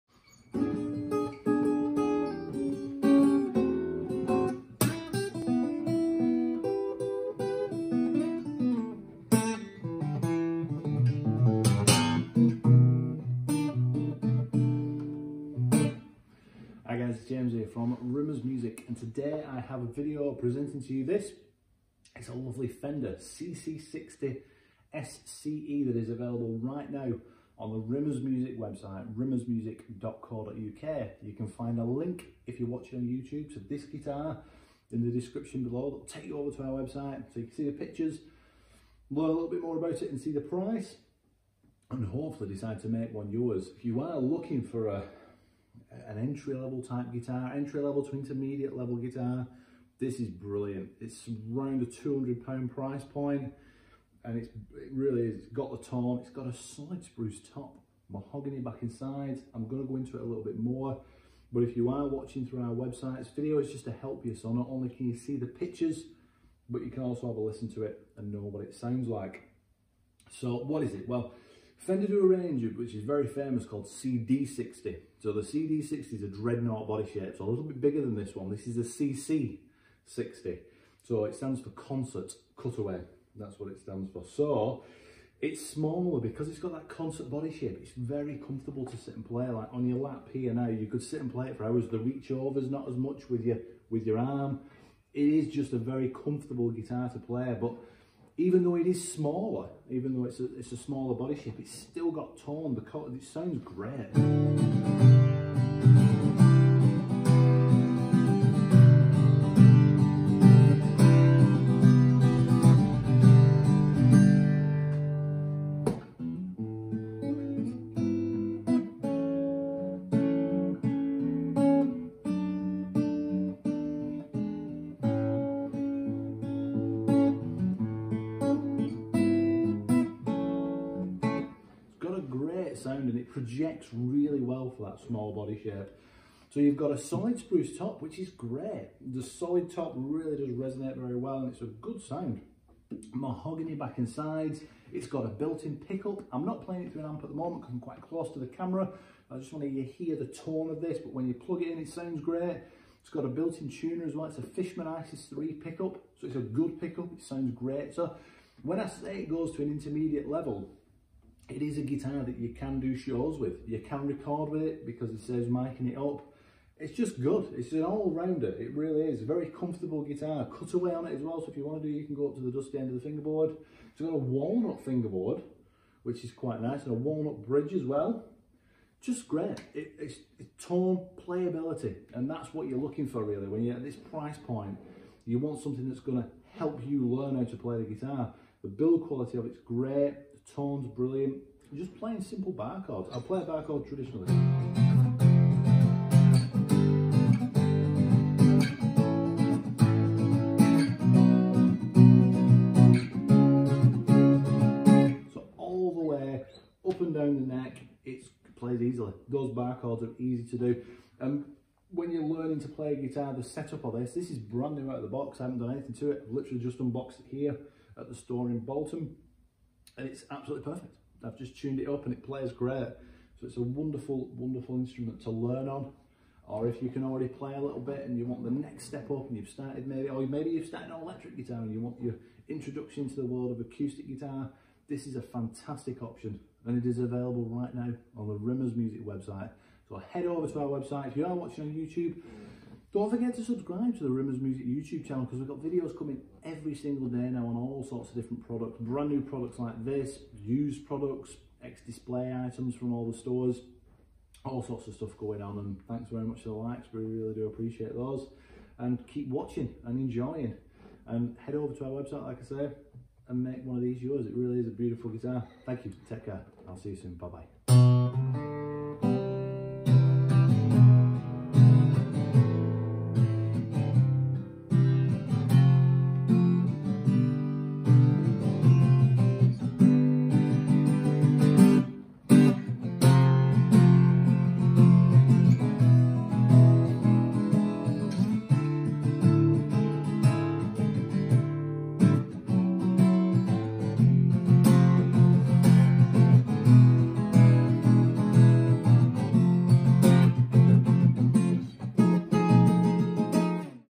Fender CC-60SCE Electro Acoustic Guitar Product Demonstration And Reasons To Buy From Rimmers Music
Here we have James from Rimmers Music demonstrating this lovely Fender CC-60SCE Electro Acoustic Guitar that is available to buy right now on the Rimmers Music website. Use the link above and head over to our website to see the fantastic price of this guitar. The guitar is great for all styles of playing and sounds lovely. The concert body shape makes it a joy to play as well.